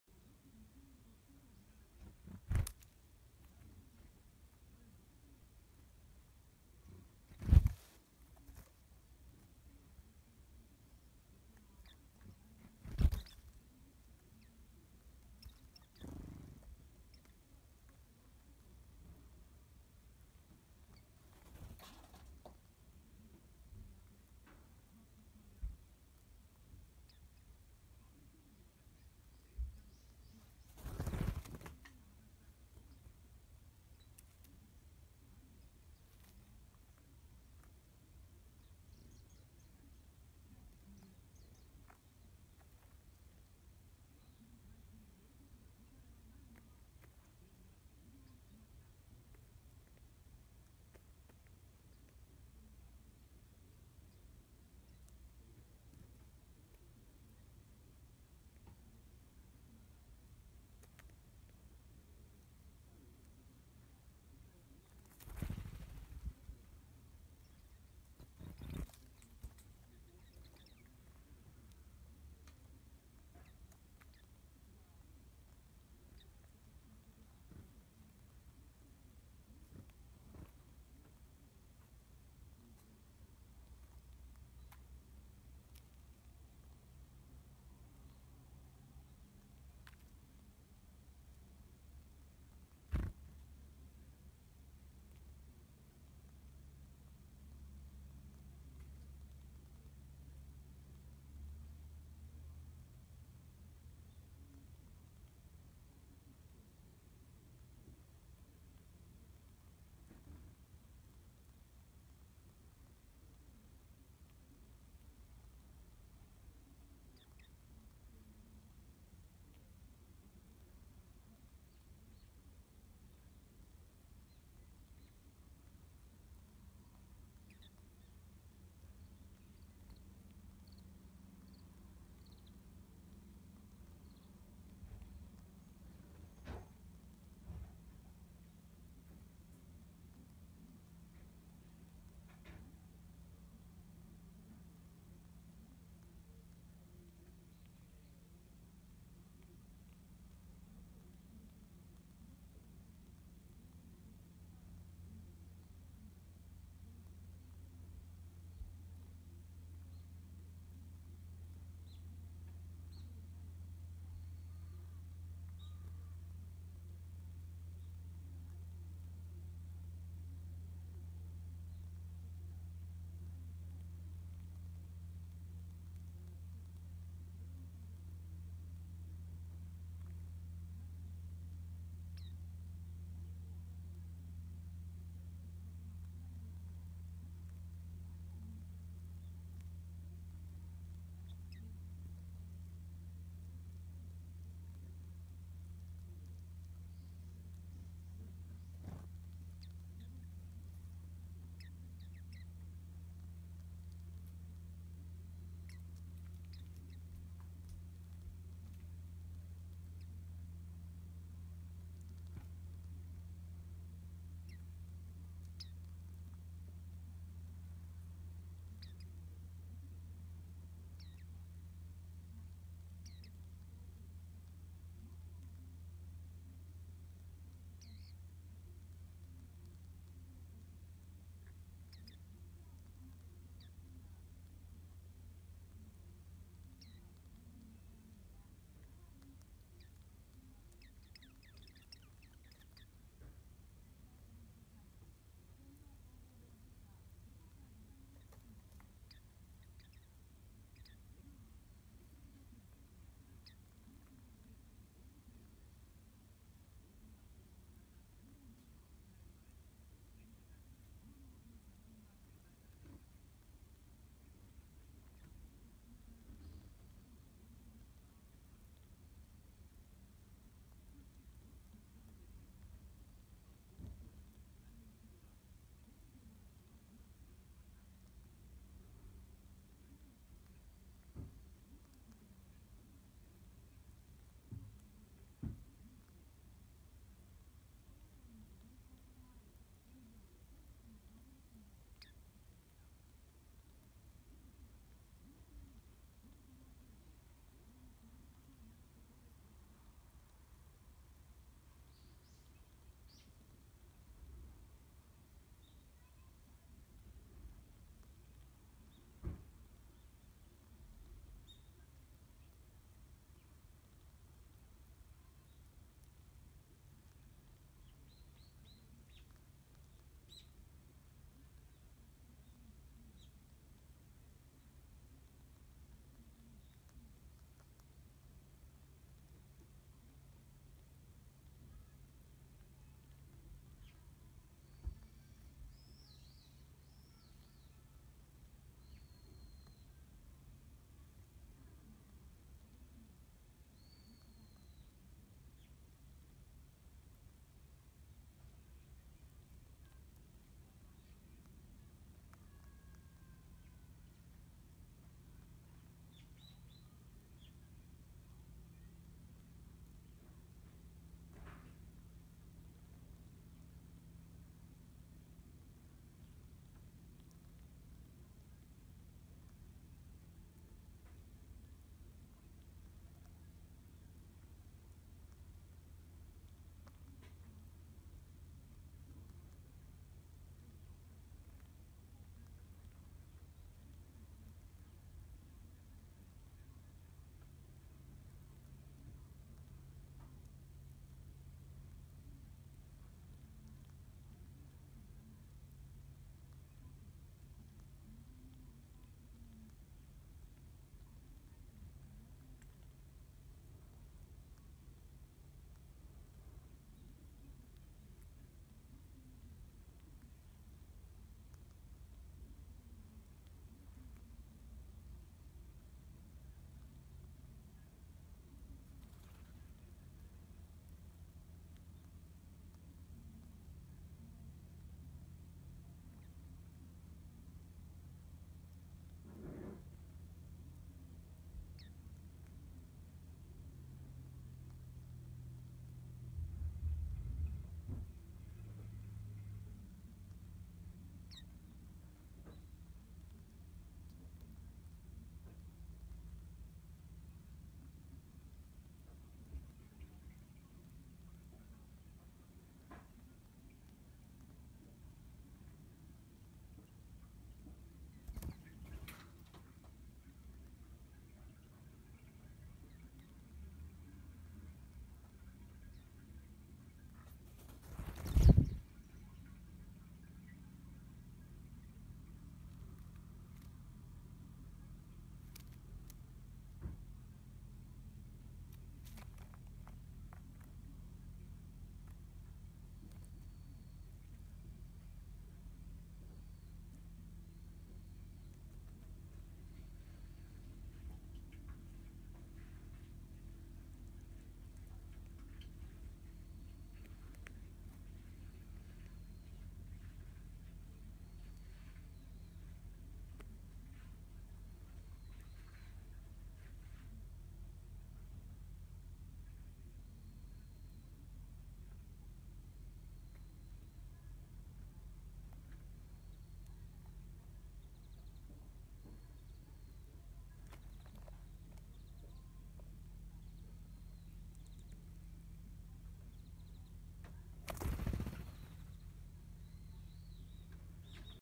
ウグイス、メジロ、ヒヨドリ。朝6時22分です。
2017年3月11日午前6時22分。朝から茂みの中が賑やかです。ウグイス、メジロ、ヒヨドリと短時間にみんなやって来てました。